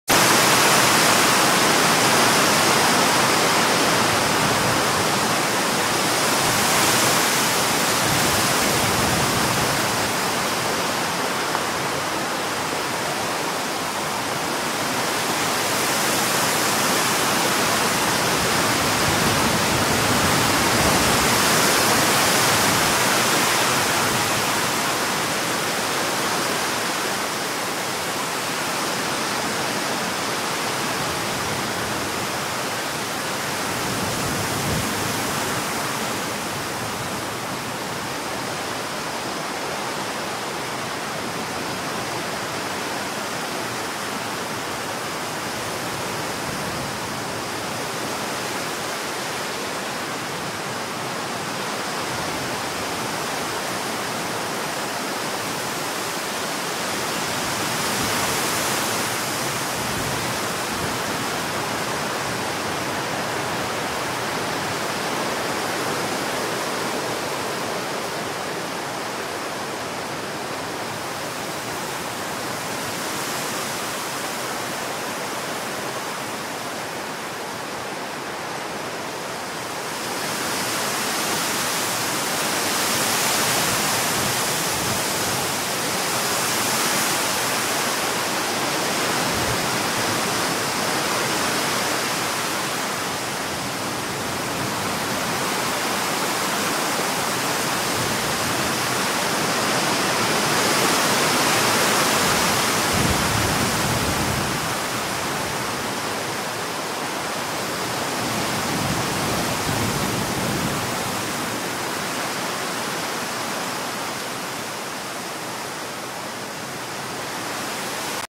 storm noise 2
Strong wind in the trees, recorded during the passage of the storm

storm sound,storm sound effects,wind sound,white noise,nature sounds,storm sound

credit sardin